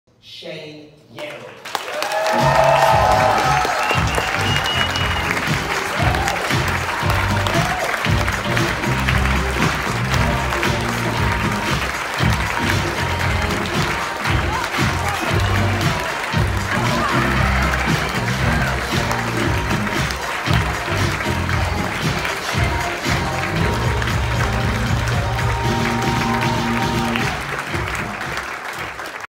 Shane Yerrell wins at JustGiving Awards 2019
Shane Yerrell receives a special recognition award at the JustGiving Awards 2019.